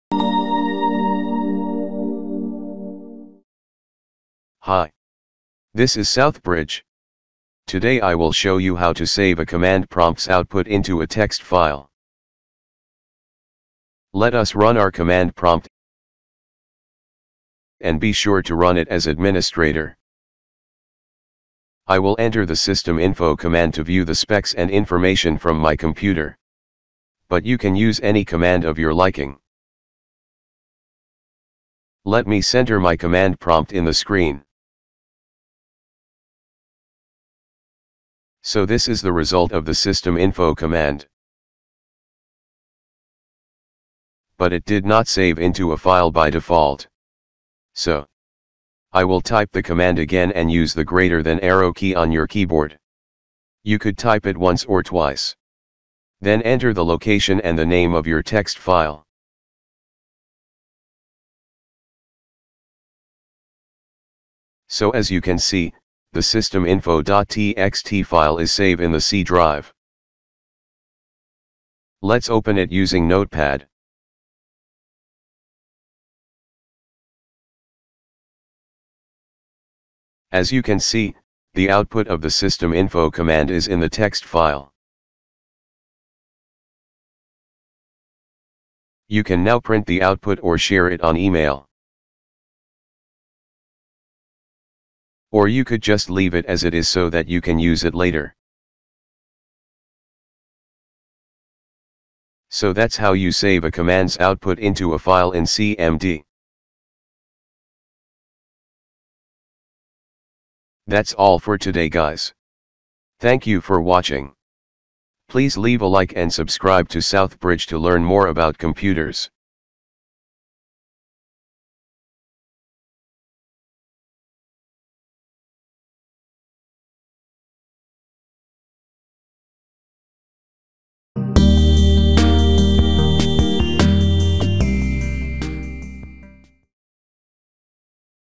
Command Prompt Tutorial - How to Save Command's Output to a File
In this video I will show you how to Save Command's Output to a File.

On Windows 10, the ability to save the output of the Command Prompt
command to a text file can come in handy in many scenarios.
For example, when you're troubleshooting a problem, it's a convenient way to export
and analyze the command output, or you can share the output with someone else
who can help. You can print a command output to a text file to save system configurations
for documentation purposes, and more.